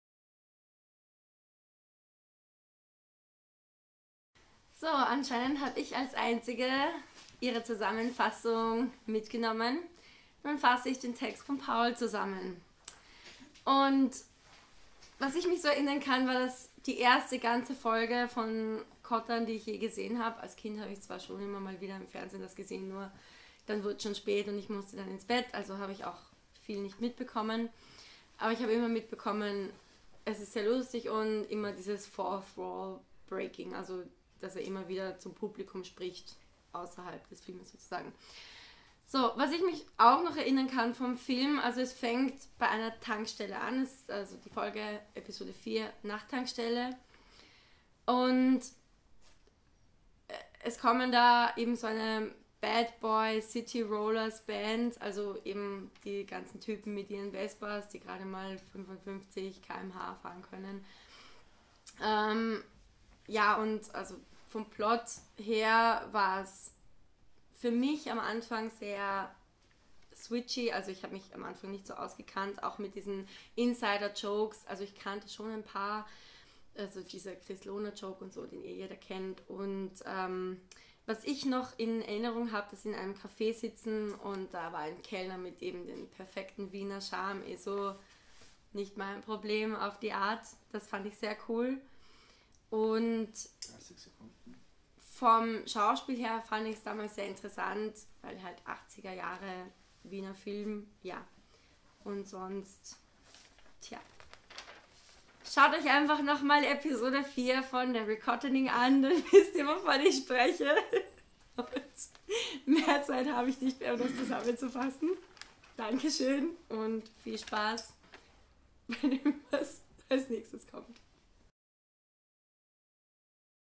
The Final Rekottaning - Recap - Simone
Simone musste sich unter verschärften Bedingungen erinnern, ihre Notizen fehlen und muss sich mit meiner Sauklaue begnügen. Trotzdem kann Simone den 4. Fall souverän lösen.